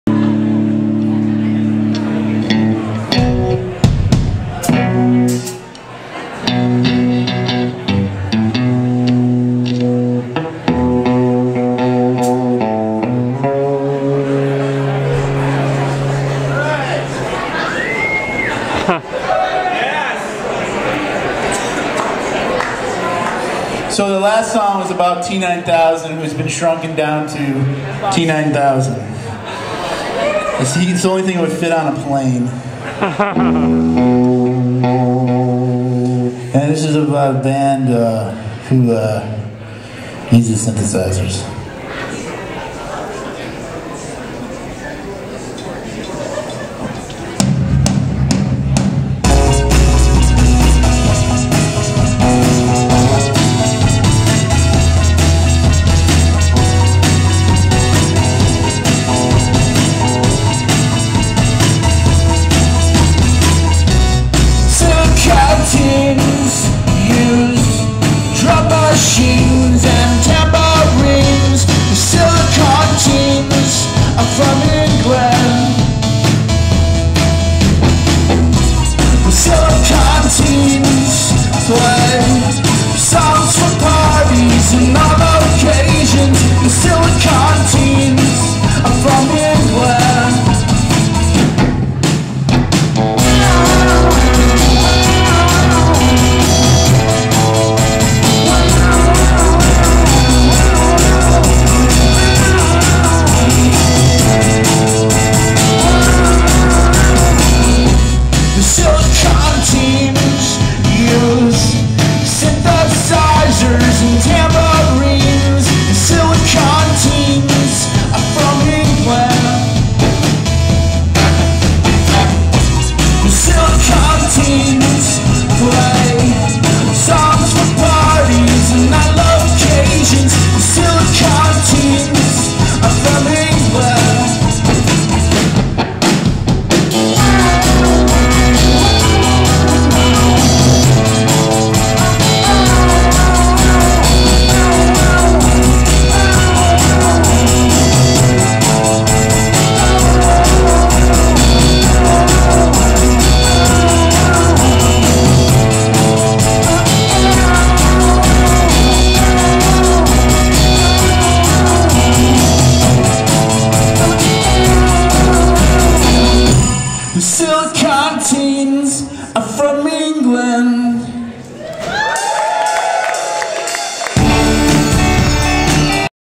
Pulsars Silicon Teens Bell House MVI_0769.AVI
Pulsars Silicon Teens Bell House live 3 DEC 2009 Brooklyn NY USA